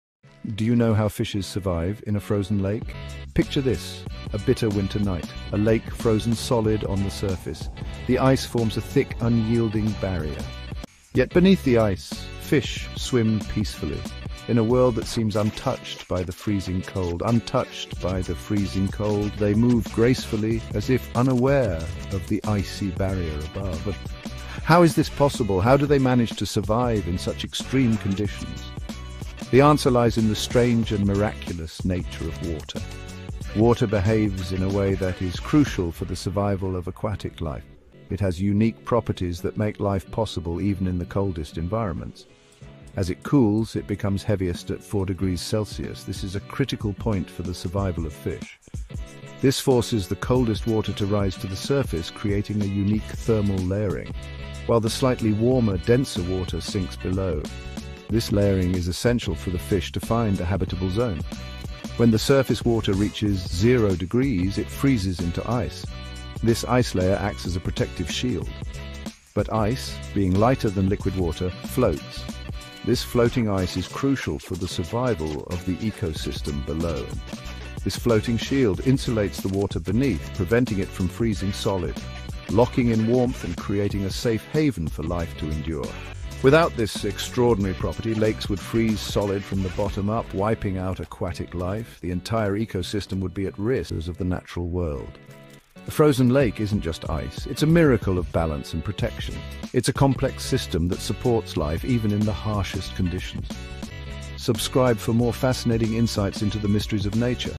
How Fish Survive in a Frozen Lake – The Science Behind Nature’s Miracle
Ever wondered how fish survive beneath a frozen lake? ❄️ While the surface turns to ice, the water below remains liquid, allowing life to thrive. This happens because water is densest at 4°C, causing warmer water to sink and ice to float, insulating the depths below. Without this unique property, lakes would freeze solid, wiping out aquatic life! Nature truly works in incredible ways.

Stay curious and explore more wonders of the natural world!